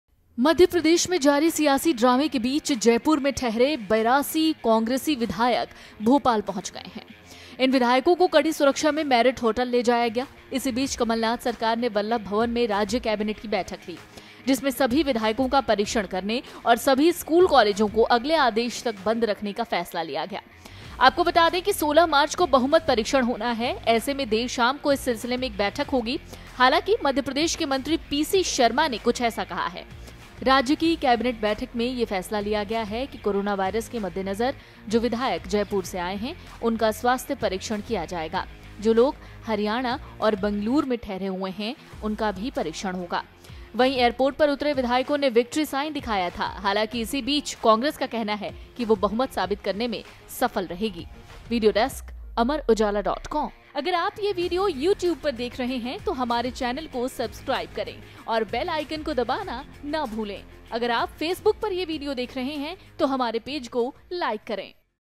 Madhya Pradesh Floor Test से पहले MLAs का होगा स्वास्थ्य परीक्षण, सियासी तूफान के बीच Corona का डर
Madhya Pradesh में Political Crisis के बीच Corona का डर भी फैला है। 16 March को Governor ने Floor Test कराने का आदेश दिया है। हालांकि इससे पहले अलग-अलग शहरों से आने वाले MLAs का Corona Test किया जाएगा।